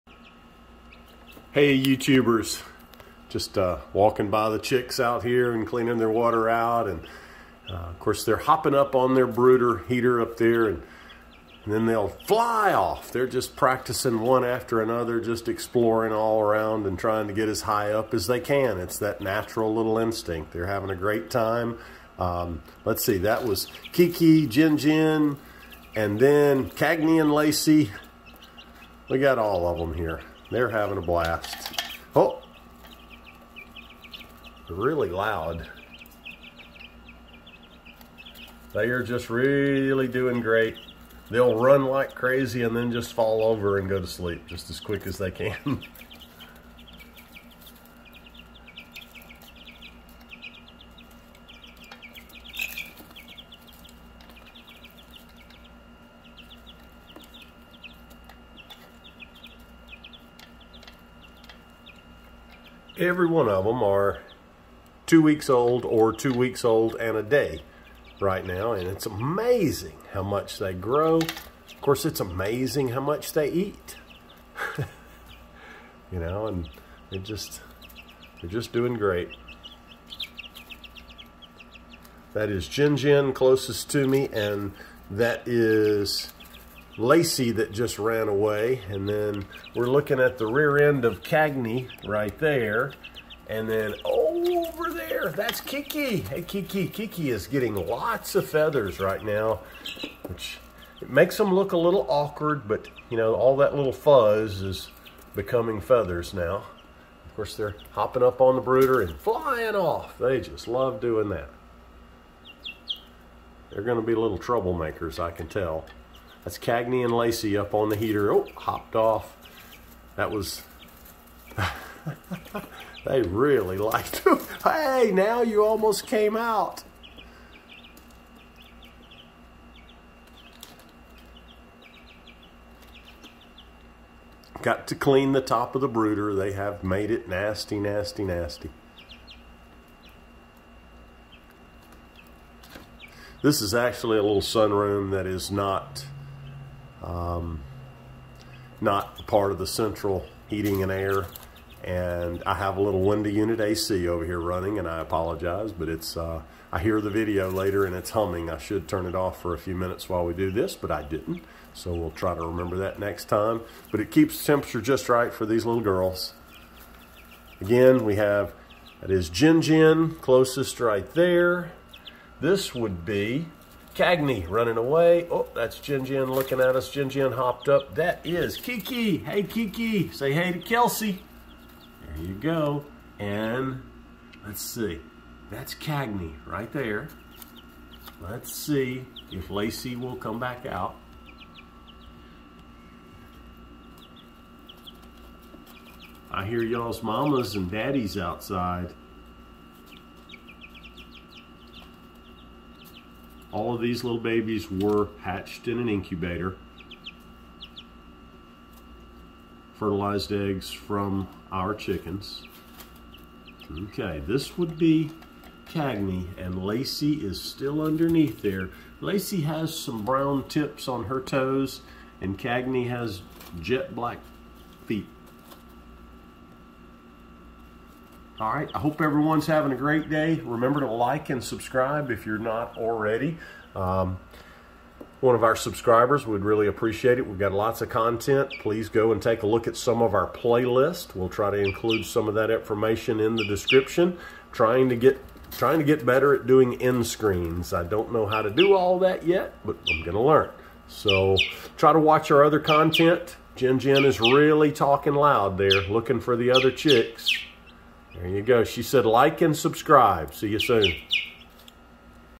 The Babies Are Practicing Flying?!! Two week Old Baby Chicks!
The babies are growing up!  Flying lessons today!

Jen-Jen

Kiki

Cagney

Lacey

Jim
Tennessee-Farm